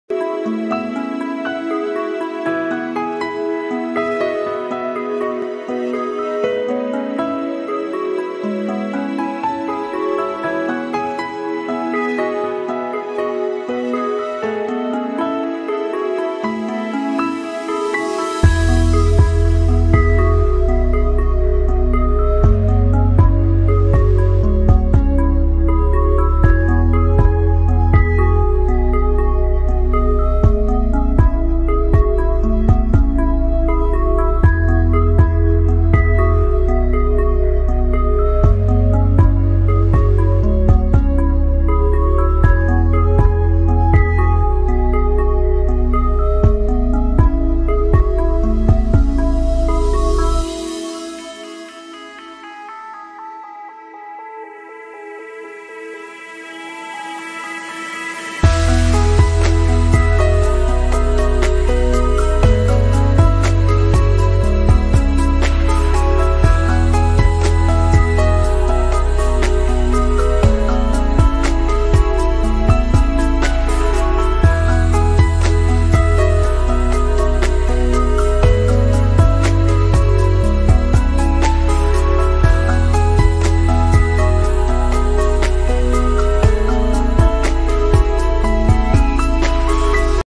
Labrador Dog Loves Sitting On Chair | Kabir - Gud Boi.
Kabir says: I'm a playful and mischievous labrador. I love to sit on a chair. I keep myself engaged in looking at all the persons passing from the gate. People love me when they see me sitting on chair. People pet me and all kids from upper storey come to meet me.